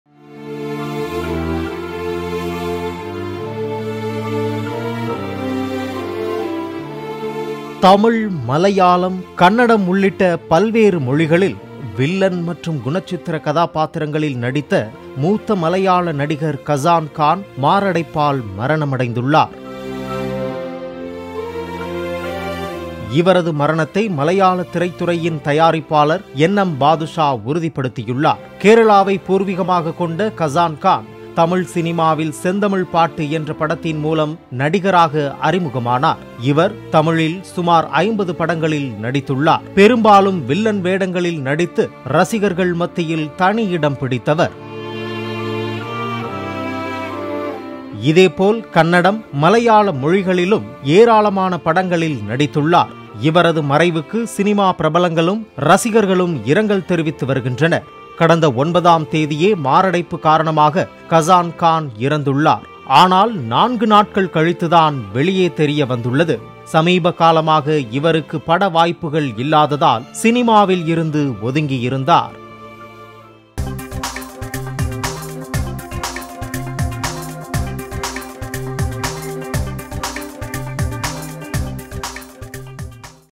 9 ஆம் தேதியே மாரடைப்பால் மரணம்... நடிகர் கசன்கான் இறந்து கூட தெரியாத சோகம்..! Kazan Khan | Polimer
9 ஆம் தேதியே மாரடைப்பால் மரணம்... நடிகர் கசன்கான் இறந்து கூட தெரியாத சோகம்..! | RIP Kazan Khan | Polimer News